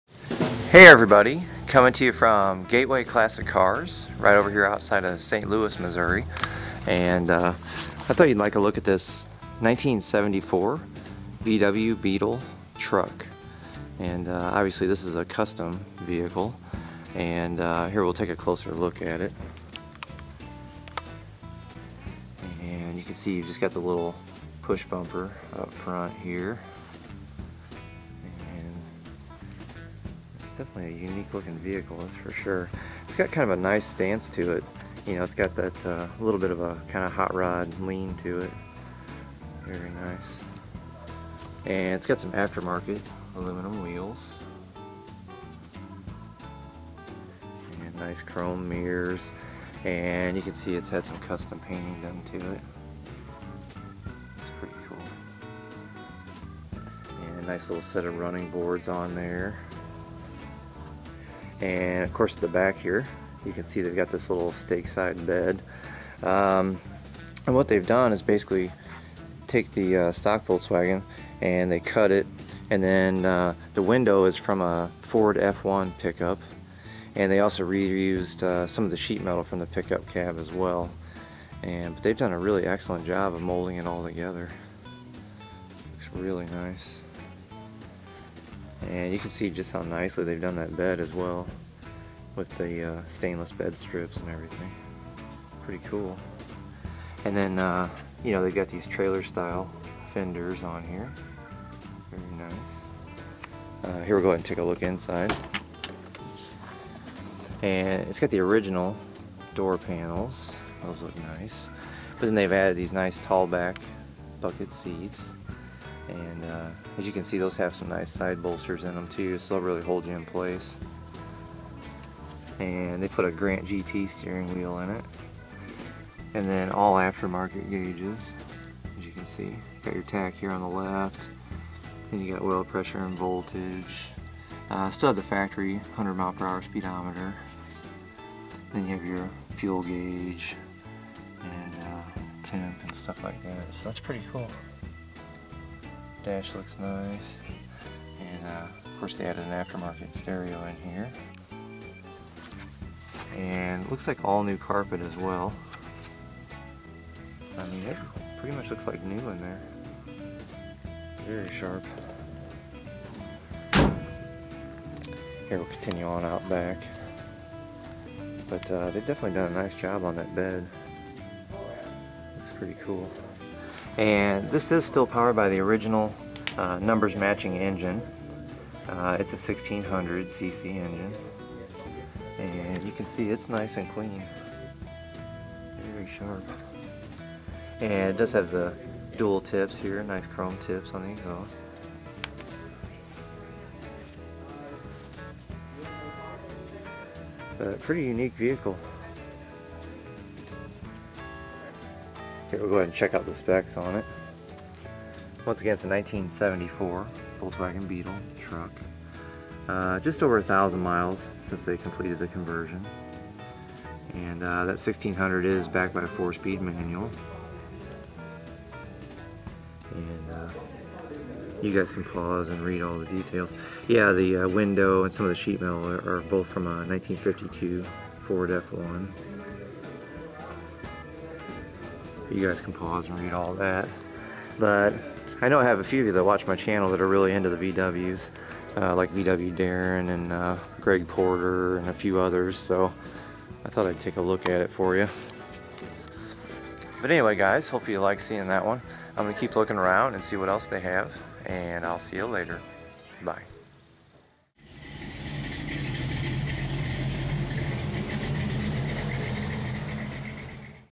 1974 VW Beetle Custom Truck
I thought you'd like a look at this custom 1974 VW Beetle Truck.  They molded in the back glass and sheetmetal from a 1952 Ford F1, to make this beetle into a pickup.  It has a custom built Oak bed with stainless steel strips.  It's still powered by the original, numbers matching 1600 cc engine backed by a four-speed manual trans.  Inside, you'll find a custom steering wheel, aftermarket gauges, and tall back bucket seats.  This is definitely a unique vehicle that will draw attention wherever you go. Thanks for taking a look!

Filmed at Gateway Classic Cars near St Louis, MO

MacLeod's description:

Genre: Blues
Length: 3:15
Instruments: Guitar, Bass, Kit, Organ, EP
Tempo: 90